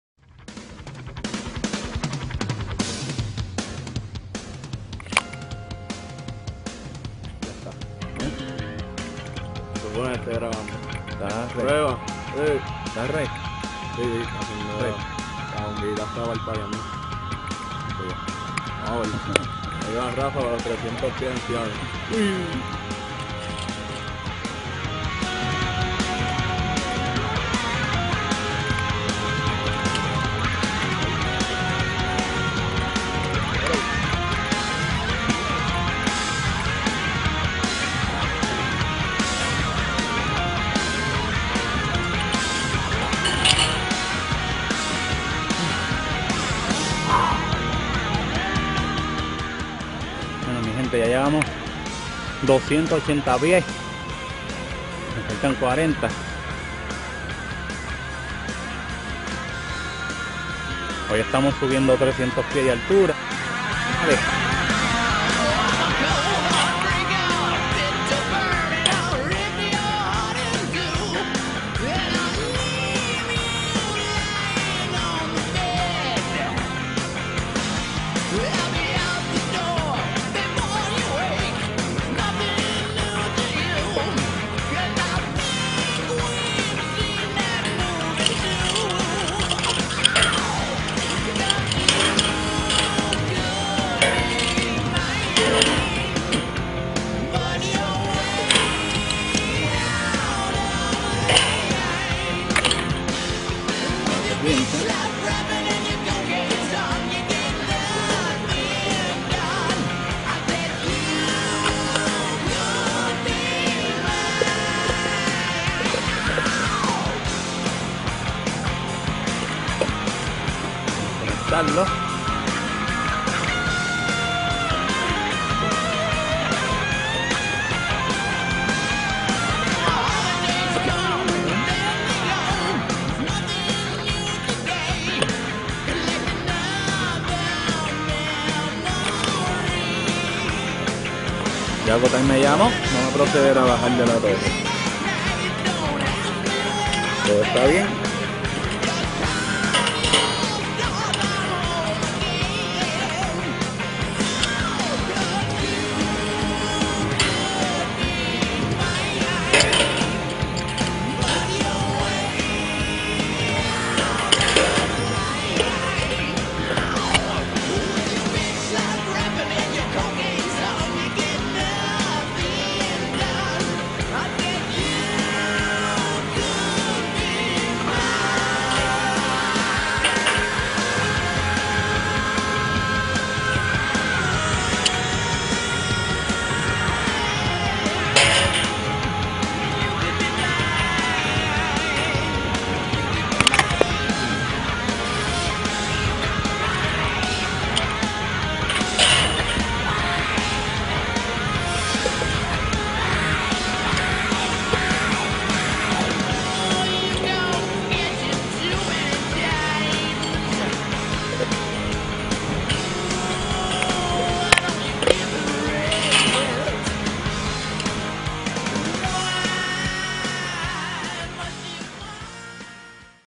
Aeronet Broadband Tower Crew Ciales - 300 ft
Aeronet Tower Crew replacing a Motorola Canopy (Cambium Networks) Microwave Backhaul unit.  Tower work was perfomed by climber Rafael Reinoso on Ciales Dowtown Tower, Puerto Rico.  Unit was a Tower top @ 300 ft.